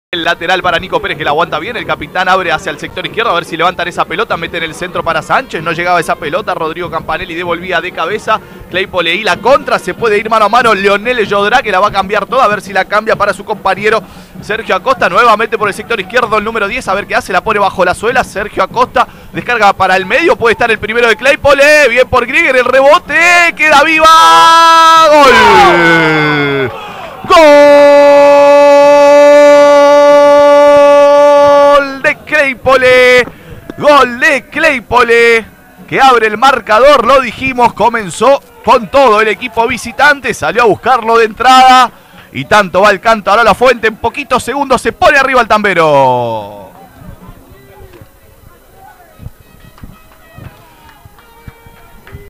Coselli ponía el 1-0 parcial de Claypole ante Atlas
📹 veo+